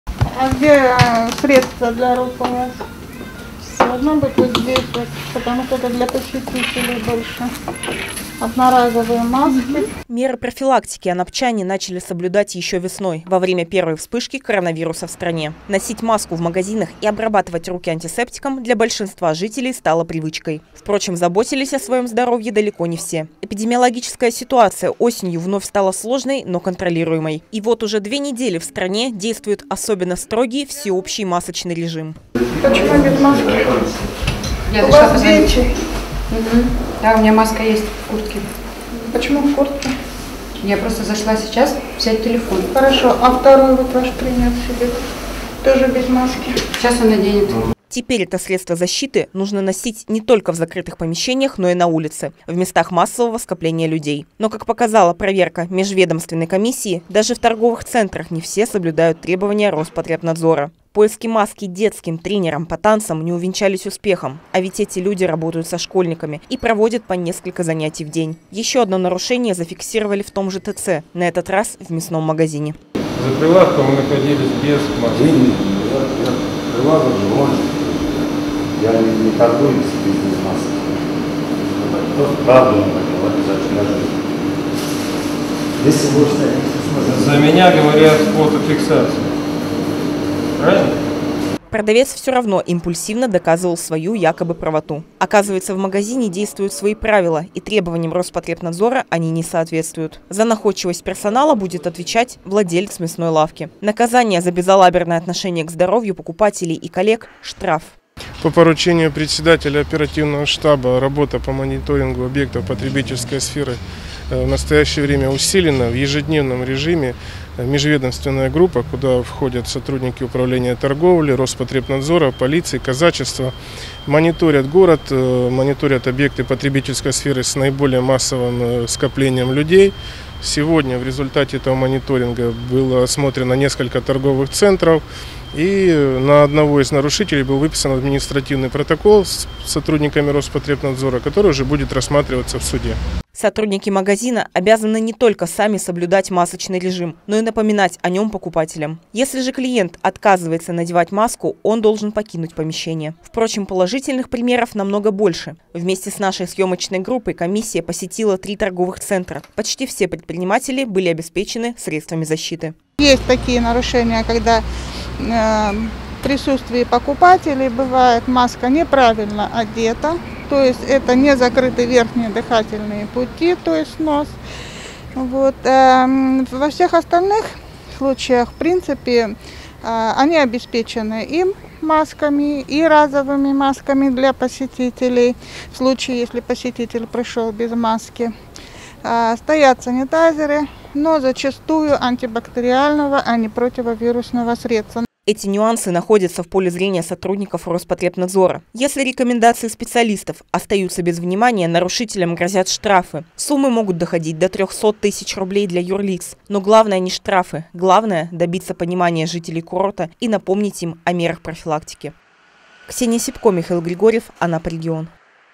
Анапские рейдовые группы усилили работу по контролю соблюдения требований Роспотребнадзора
Накануне специалисты федеральной службы, полицейские и сотрудники администрации посетили несколько популярных торговых центров. Как результат — протокол и масса предупреждений предпринимателям. А ведь статистика заболеваемости продолжает расти.